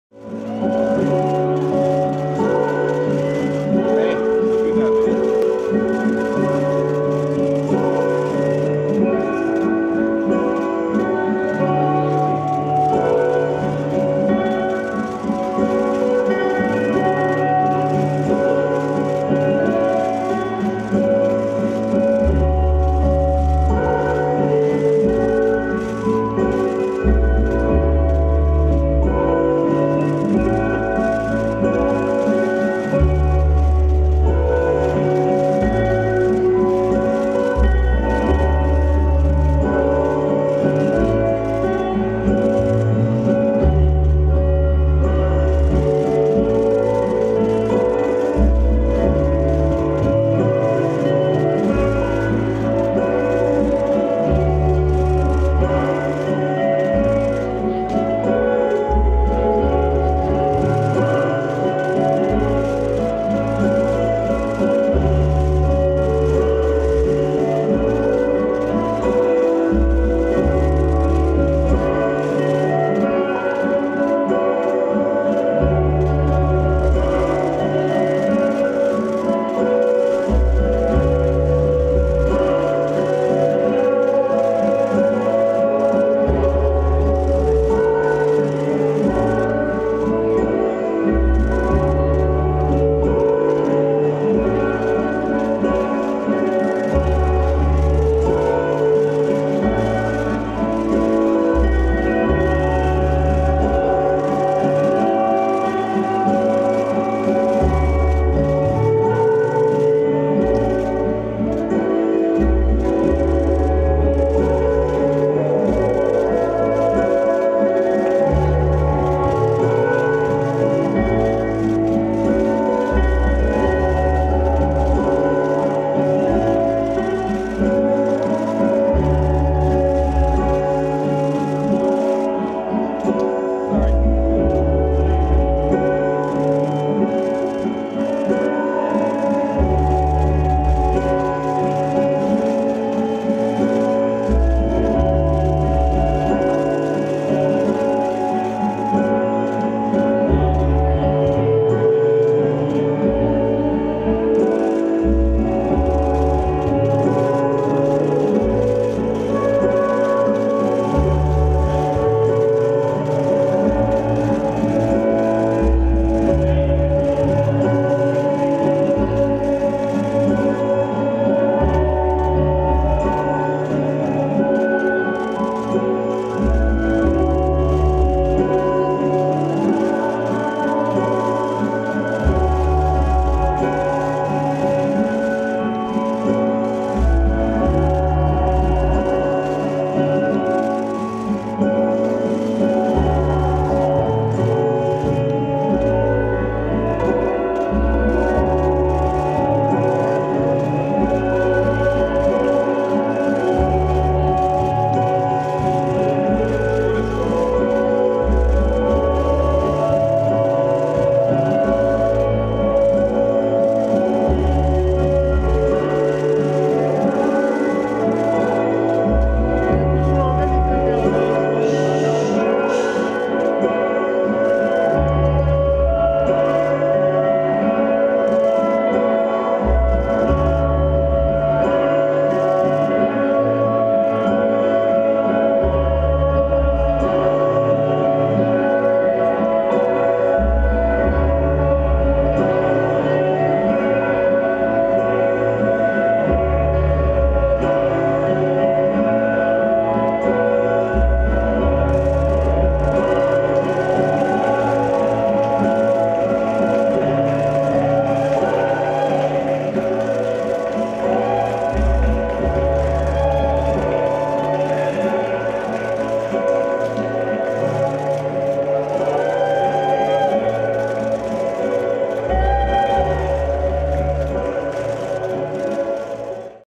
Tigers Eye Clothing at Art Hearts Fashion NYFW The Shows New York Fashion Week
Tigers Eye Clothing at ArtHeartsFashion NYFW The Shows SS/17 Presented by AIDS Healthcare Foundation.